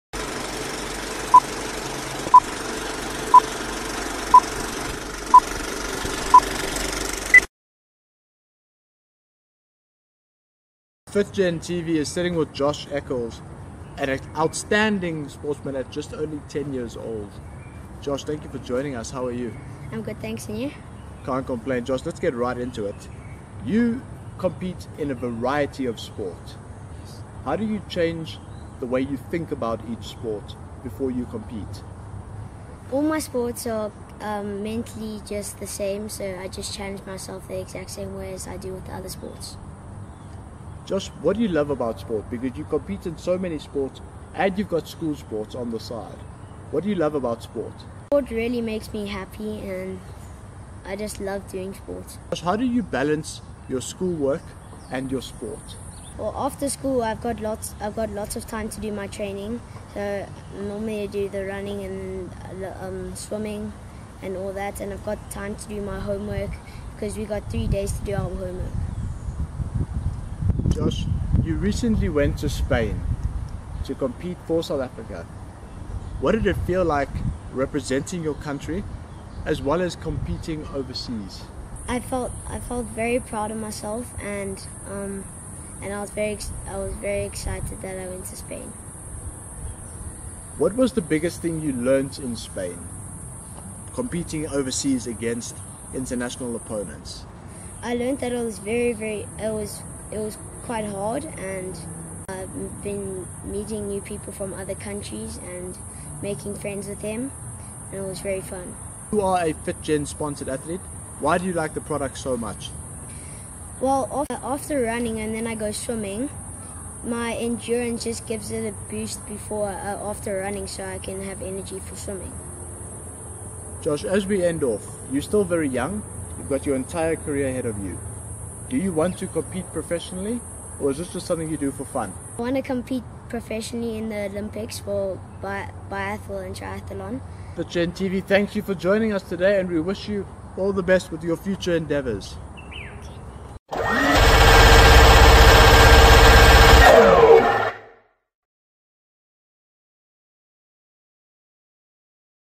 FitGen TV: Josh Eccles
FitGen TV travelled to Sandton to meet up with 10-year-old sporting sensation Josh Eccles who has had an incredible 2017 on the sporting scene.

Full name: Joshua Eccles
DoB: 27 July 2007
PoB: Sandton
Primary school: St. Stithians Boys' Prep
Favourite FitGen product/s: Protein Bars, Endurance, Compete
Disciplines: Biathle, Triathle, Laser Run, Triathlon, Swimming
Home club: Gauteng Biathle/Wahoo Swimming

Achievements:

* Won gold in Triathlon, Biathle, Triathle and Lazer Run at the Gauteng Championships and SA Championships (2017).

* Won six gold medals at the UIPM Biathle and Triathle World Championships in Spain (2017).